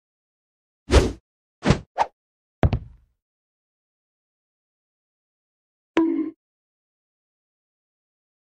Failed Serve || animation
My smoothest animation I've made so far. Took me a couple hours. Hope you enjoyed. Made in sticknodes.